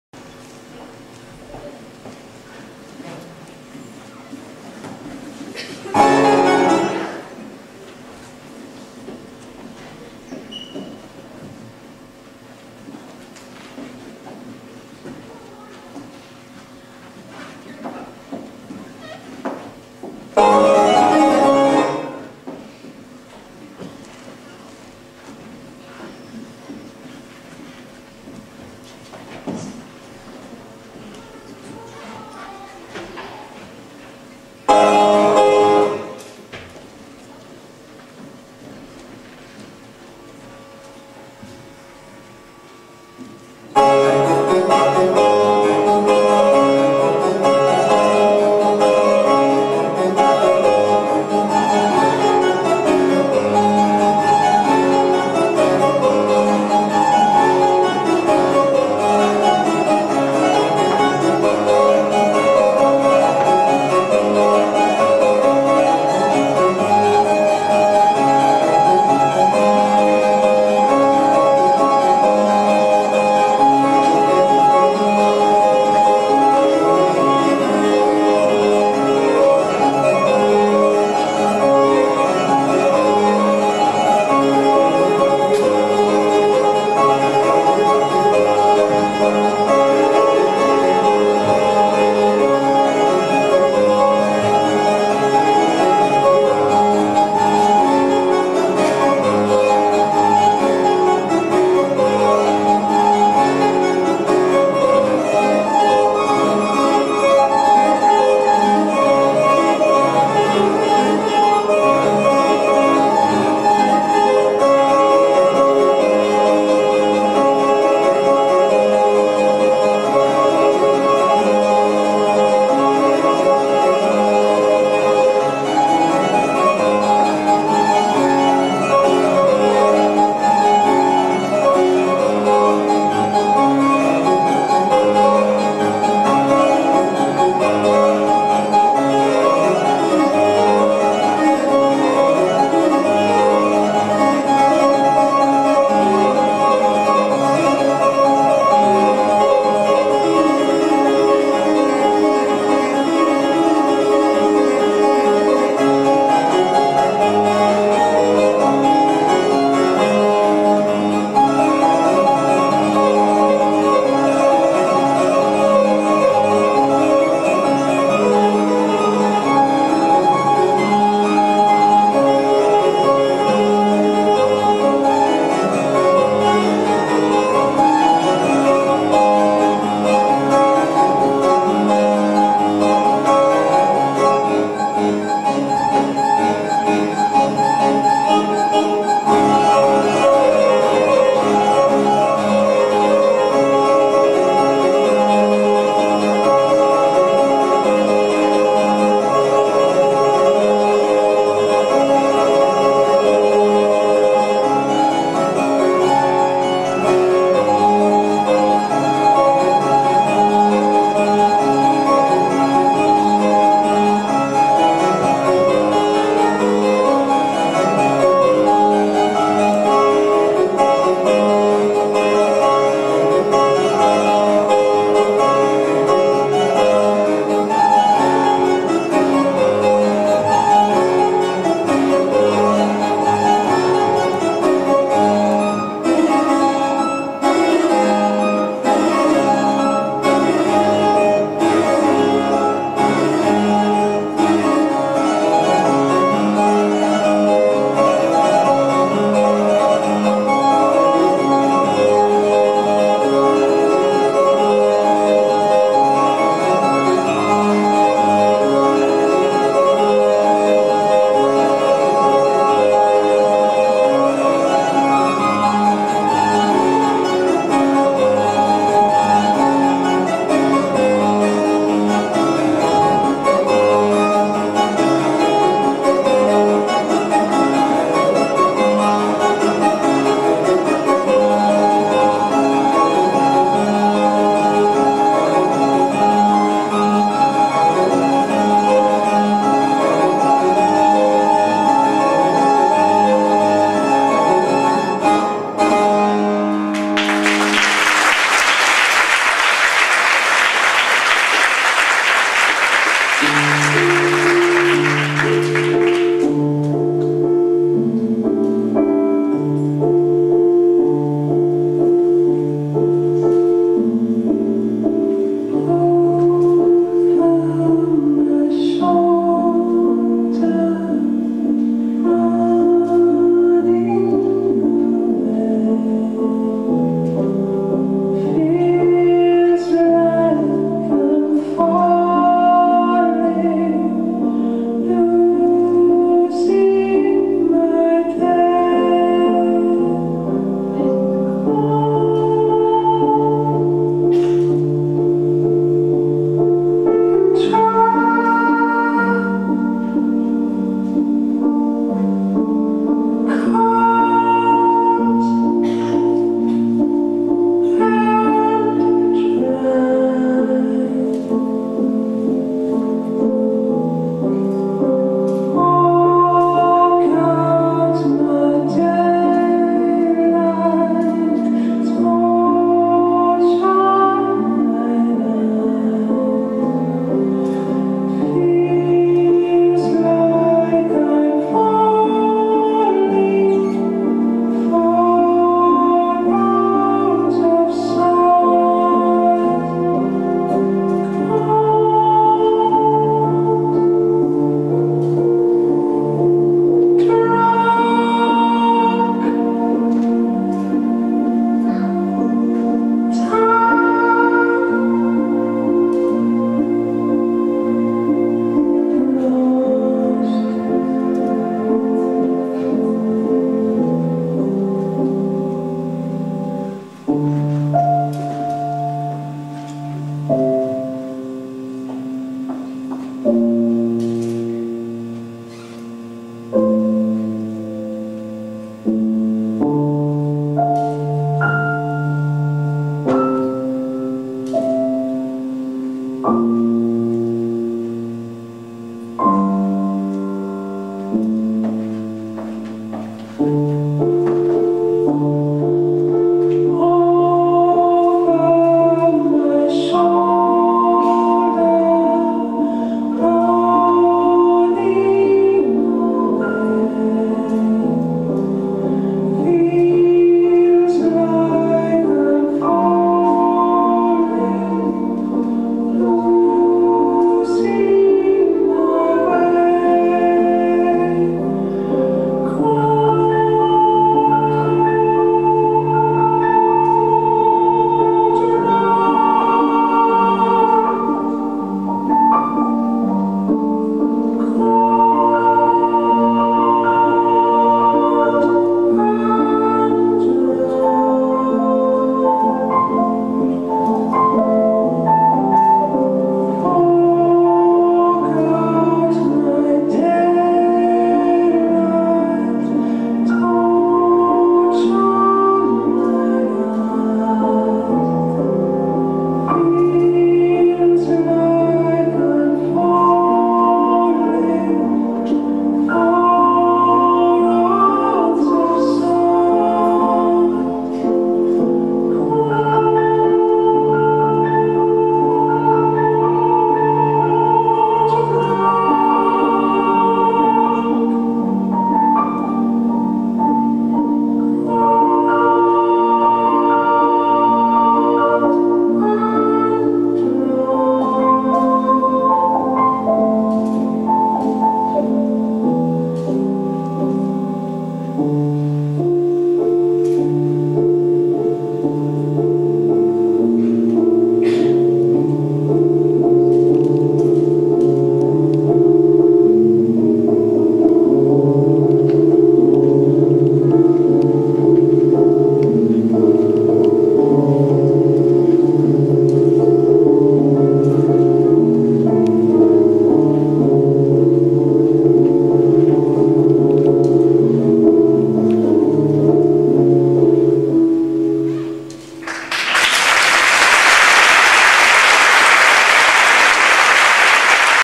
Il Balletto-Punto di partenza Saggio 2007 YAGP 2008 season
Coreografia Elias Garcia Herrera
Saggio 2007, prima presentazione
2nd prize YAGP Faenza season 2008 European Semi-finals, finalist ensemble in NY 2008
Scuola Il Balletto-Castelfranco Veneto